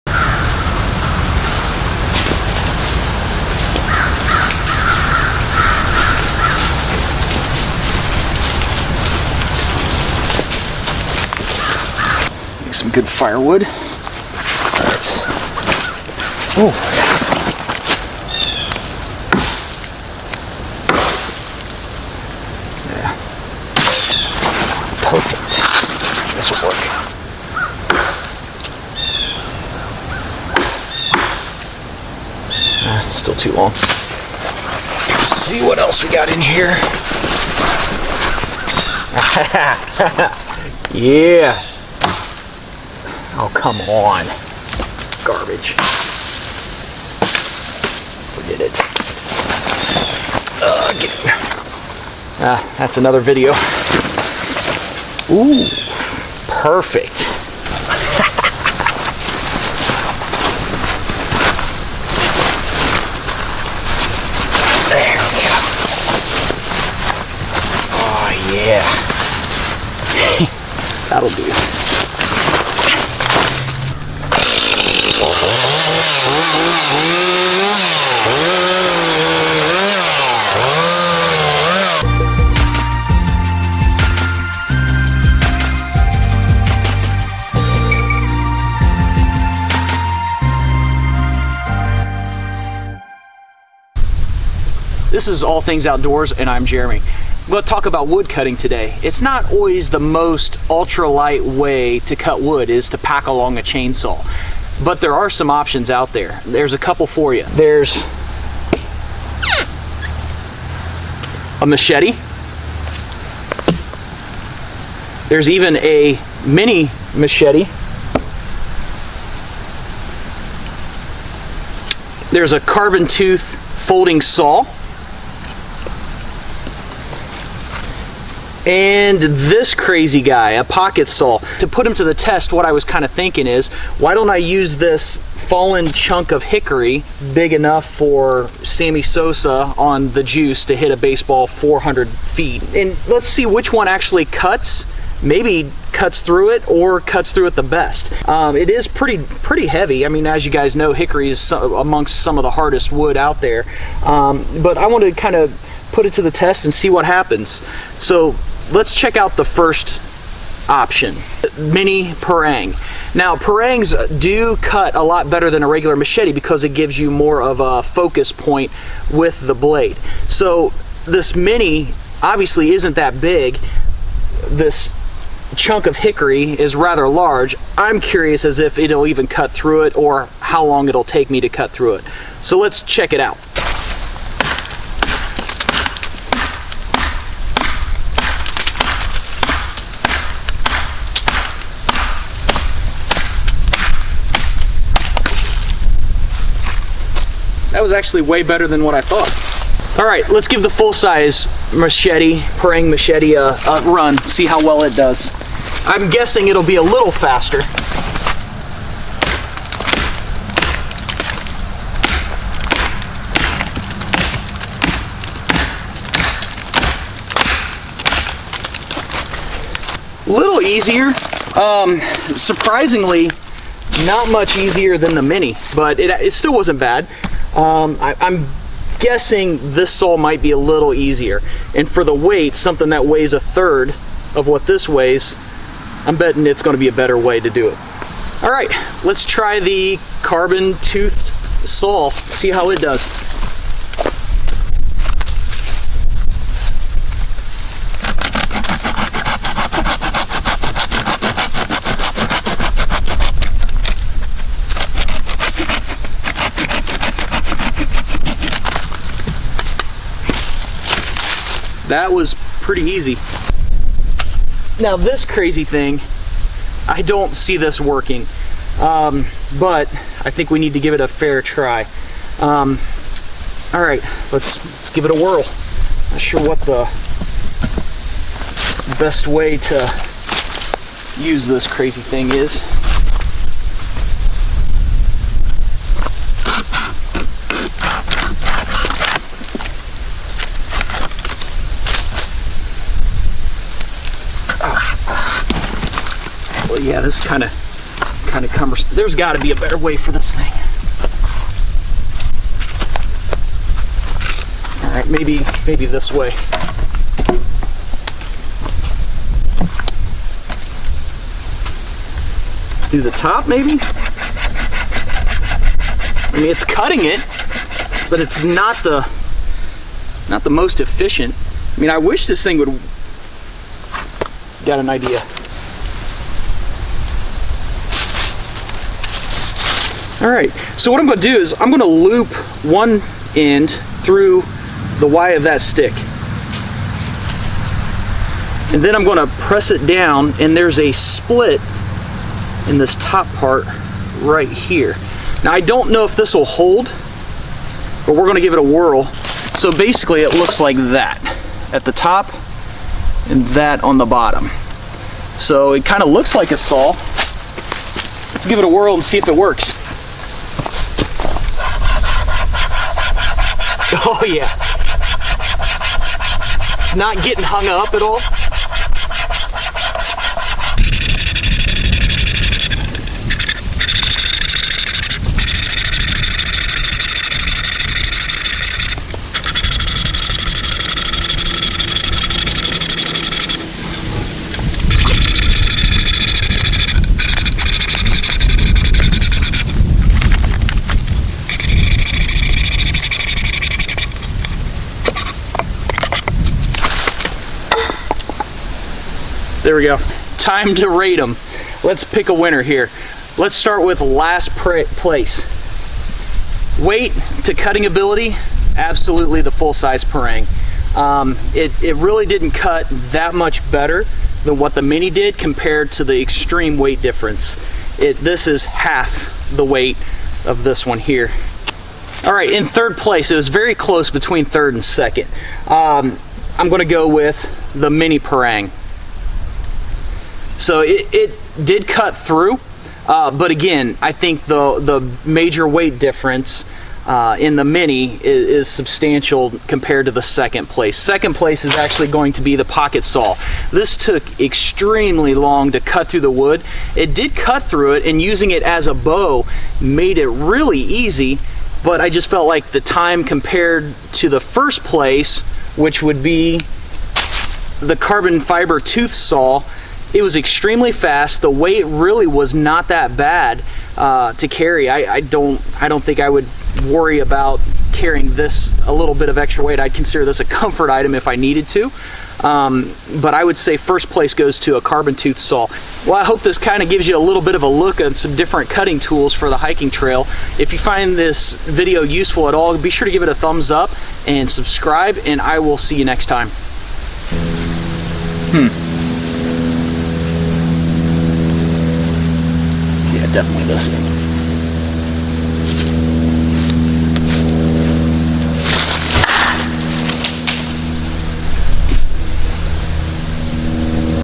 Cutting Firewood on the Trail
A look at different ways to get firewood in the backwoods. Take a look at what one I think is best. The following criteria are what based my decision (IMO) in order of importance.
1. Ease of use
2. Packability
2. Weight
3. Speed of cut.
All import factors I believe when picking a tool for a backpack.